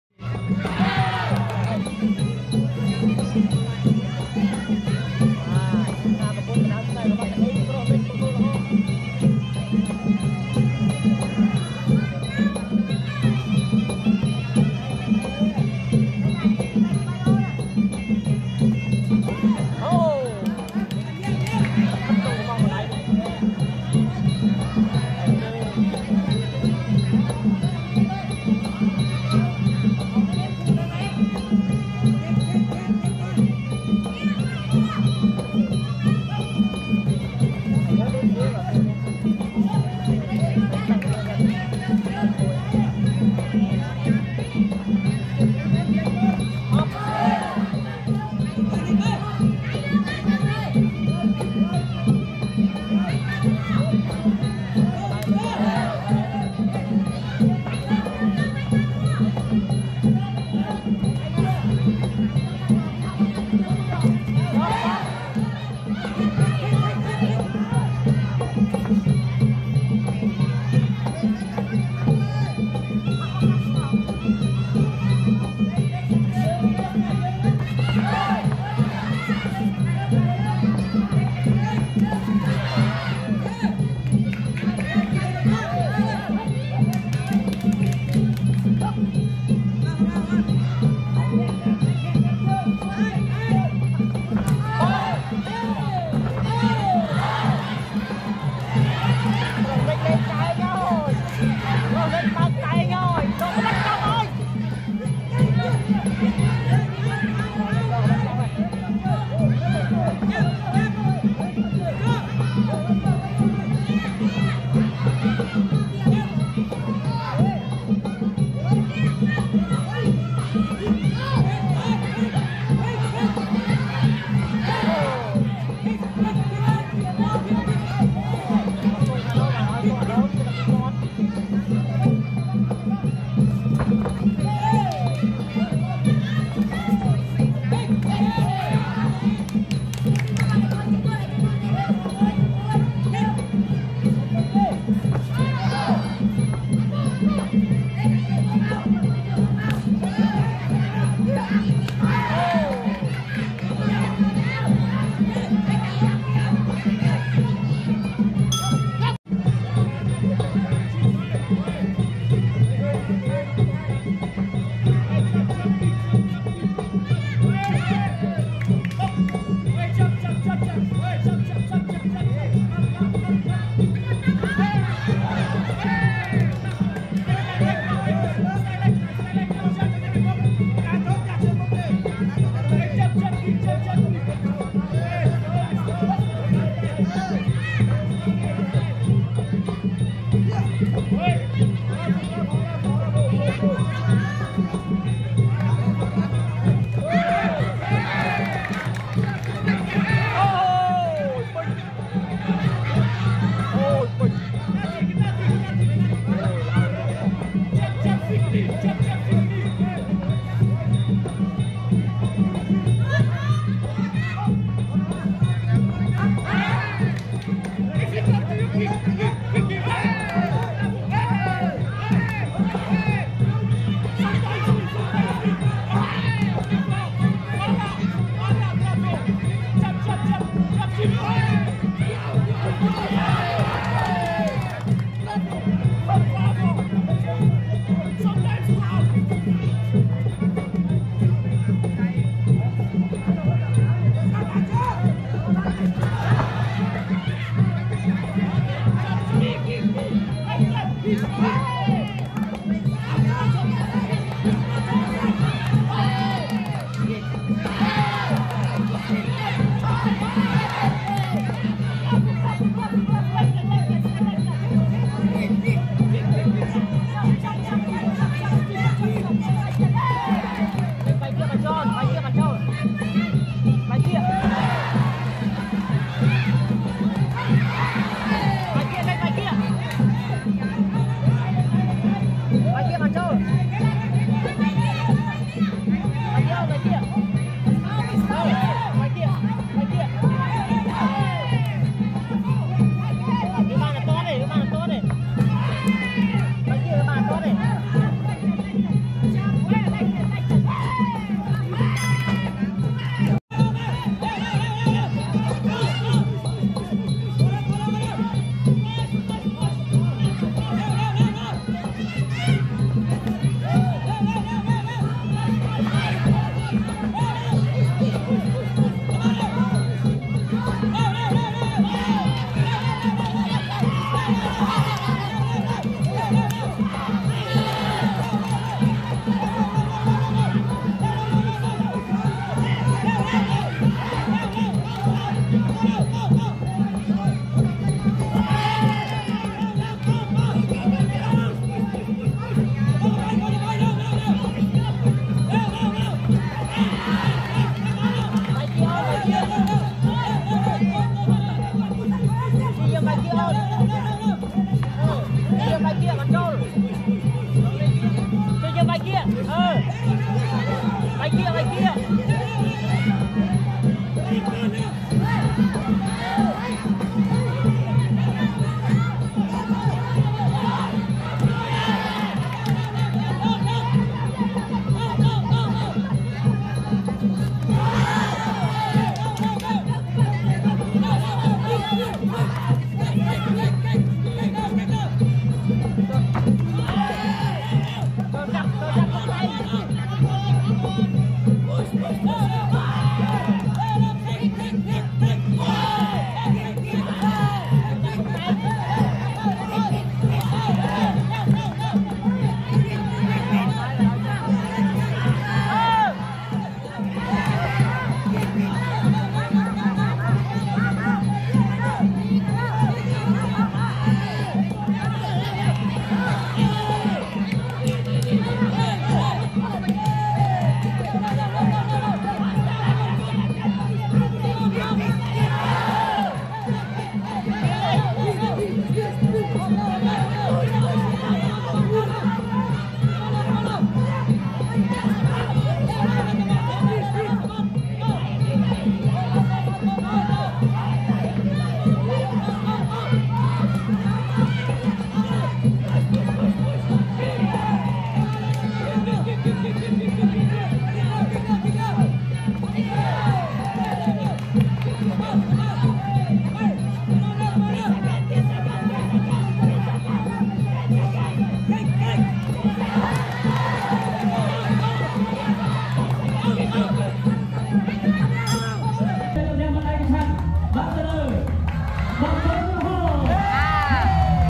ល្អណាស់ ឡើងទៅវគ្គផ្ដាច់ព្រ័ត្ន...ហេង ប្រុសនិច 🇰🇭Vs🇮🇷 យ៉ារ៉ាសម៉ាឌី អារៀន ( Iran ) 26-5-2024 Kun Khmer
Kun Khmer Kickboxing Championship